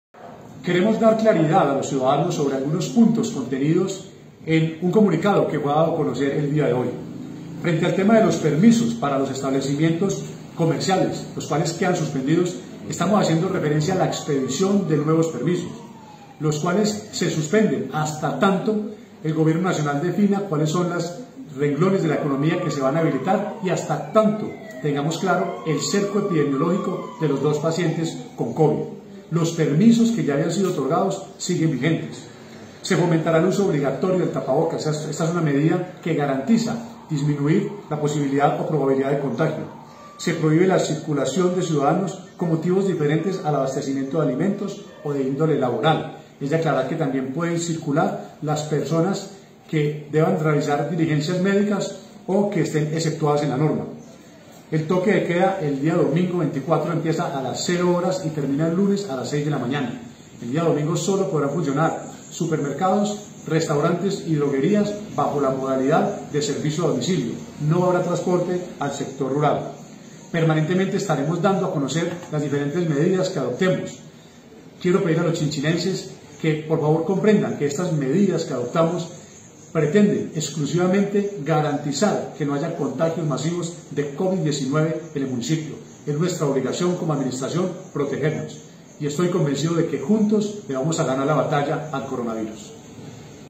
Mayo 18 aclaraciones del alcalde medidas tomadas por emergencia COVID-19
El alcalde de Chinchiná Eduardo Andrés Grisales López, le explica a la comunidad las medidas que se implementarán frente al Covid-19, durante la semana del 18 de mayo hasta el 24 de mayo del 2020, teniendo en cuenta los puntos del comunicado emitido.